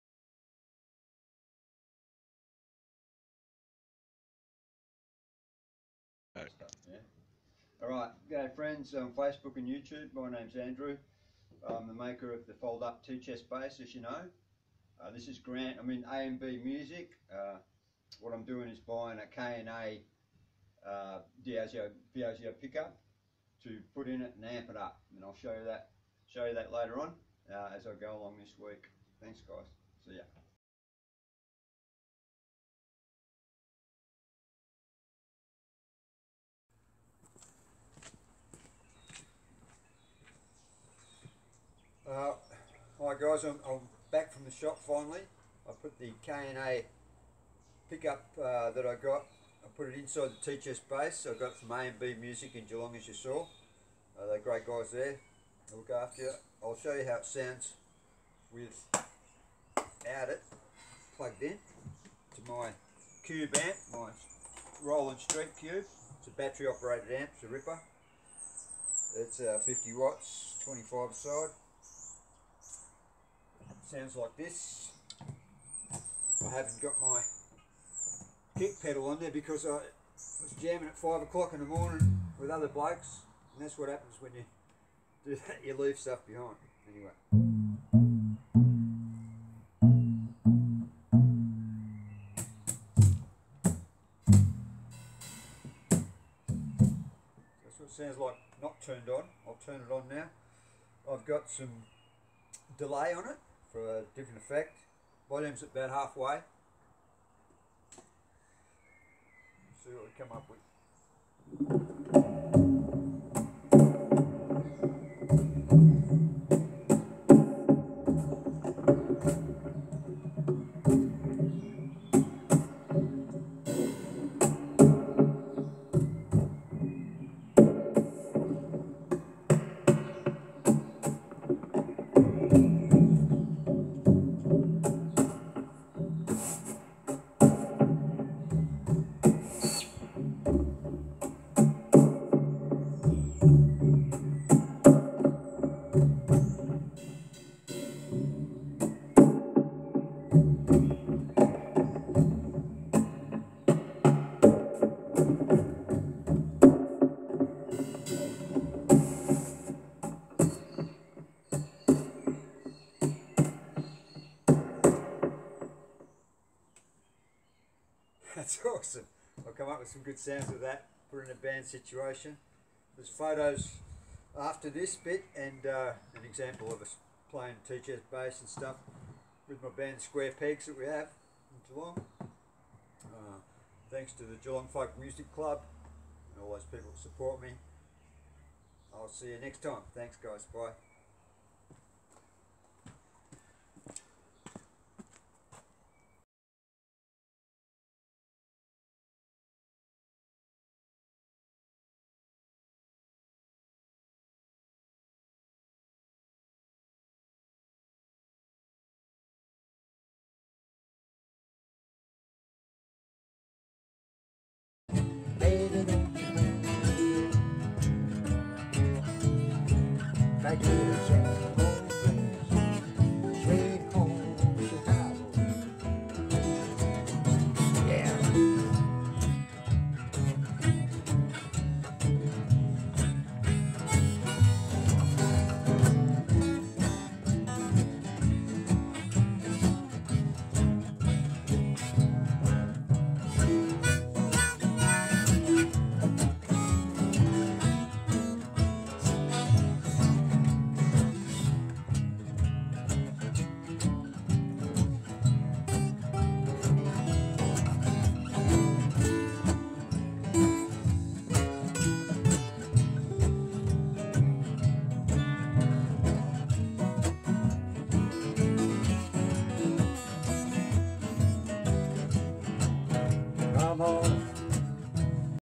" Tea Chest Bass " AMPED UP!
Placing a KNA PIEZO Electric pickup on my "Tea Chest Bass". Thanks to A&B music Geelong!
The Geelong Folk Club, and "The Square Pegs" For all their support!